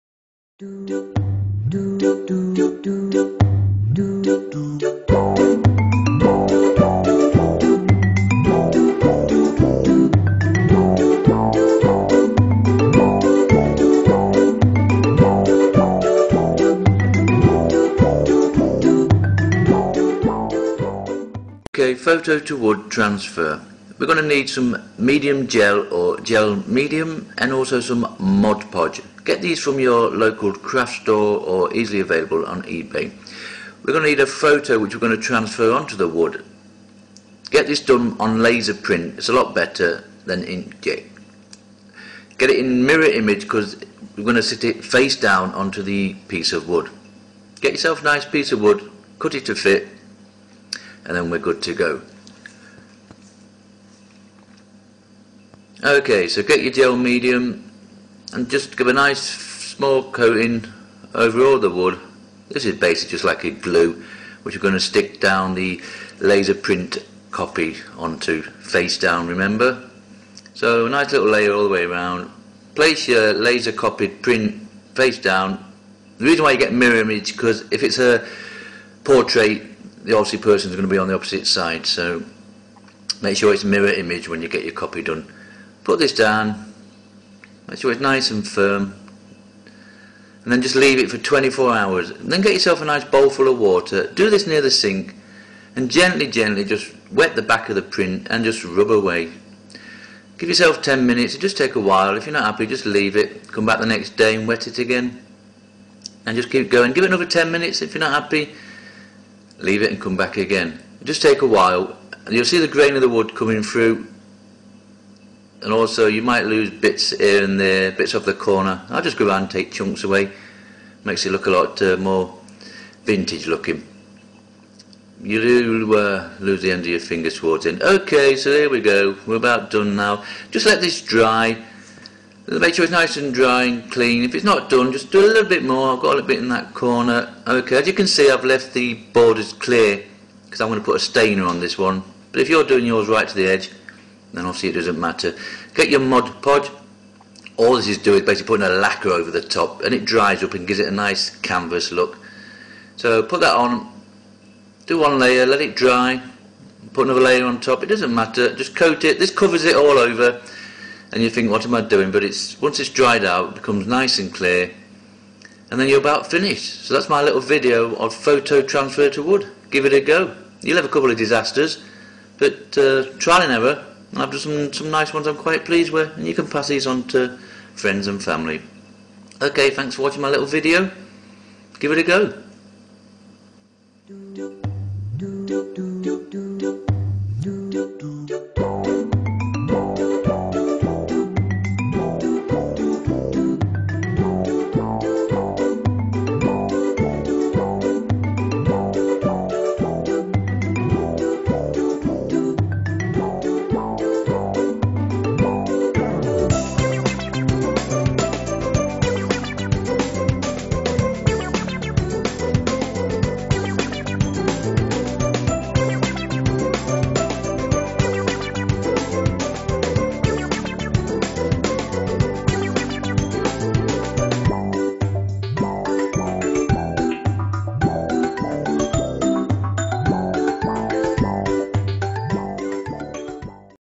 photo to wood transfer
transfer your photo to wood step by step